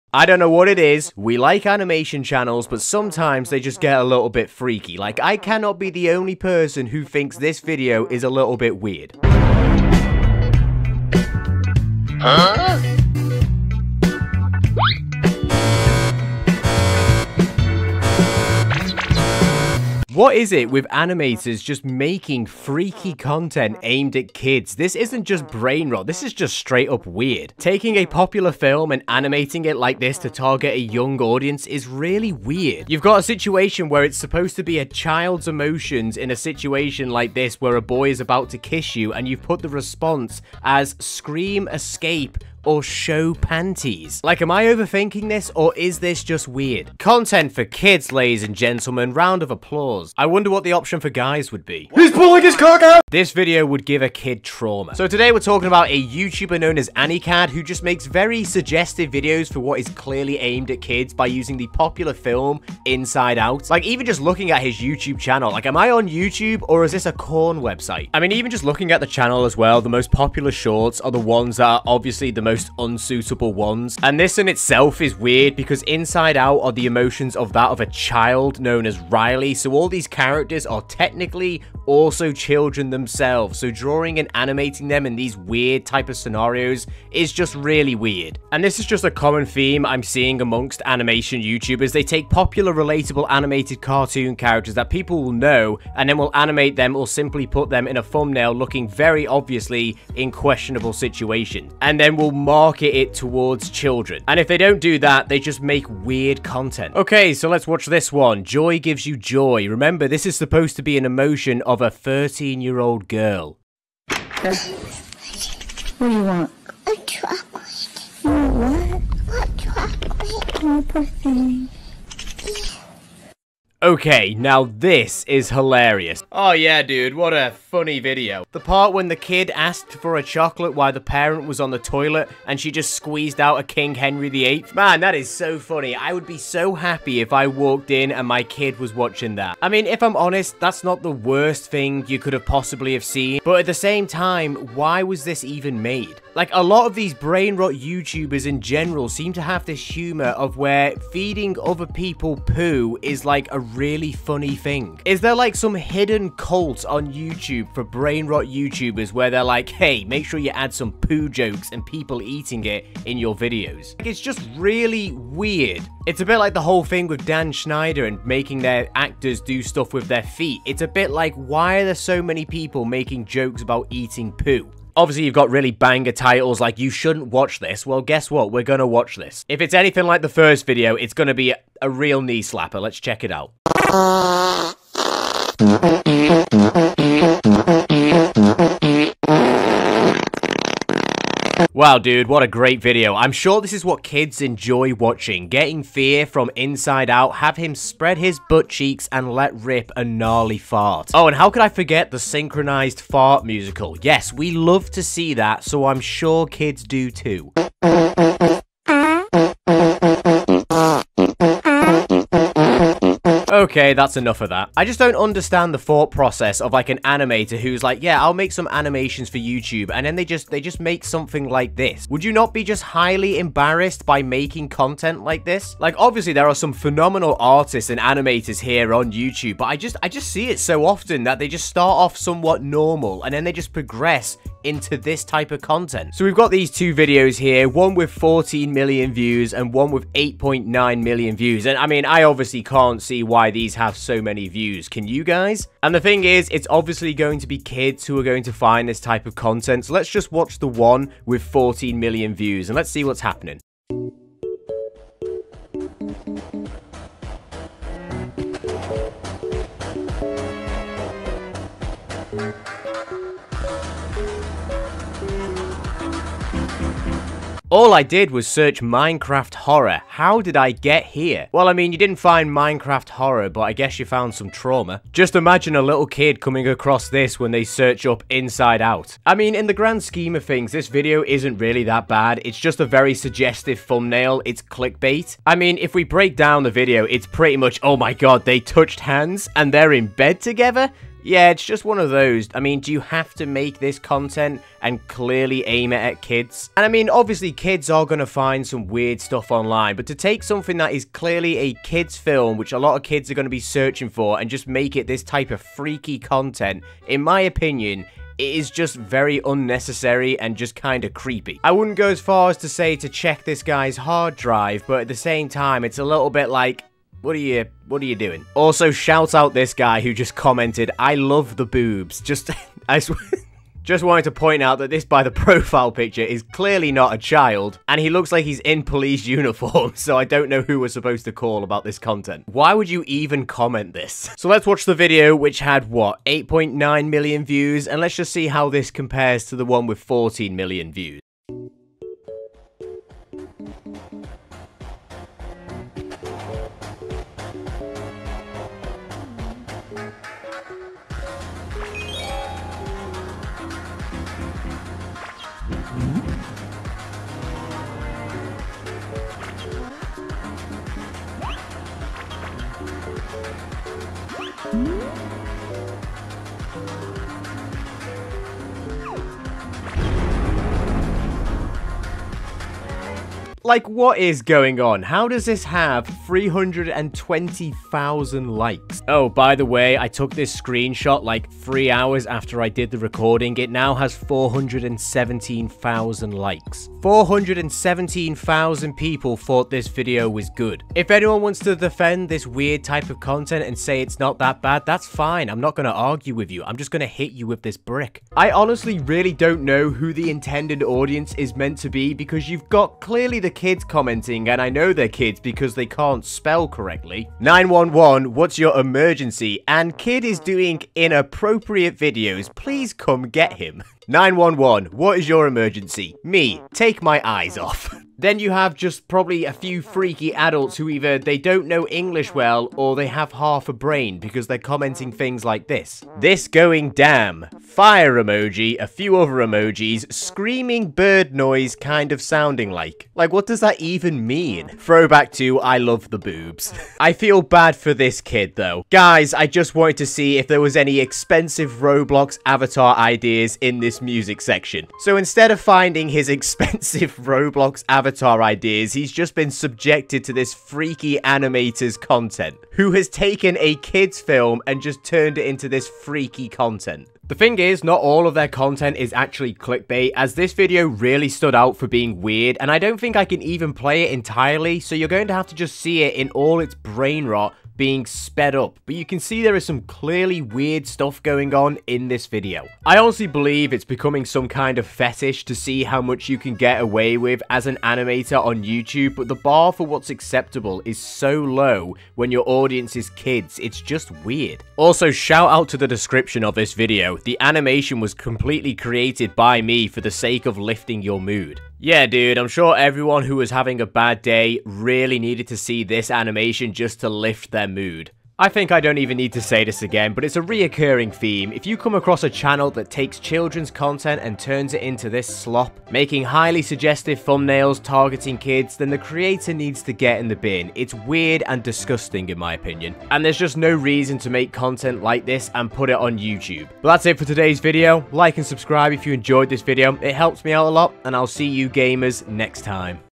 The Absolute WORST Animation Content Farms
This Brain Rot Content Farm IS DISGUSTING...Anikad is a YouTube "kid" animator who has being making some weird animations of the popular kids films Inside out 2 just like rocky rakoon his content is aimed at a younger audience..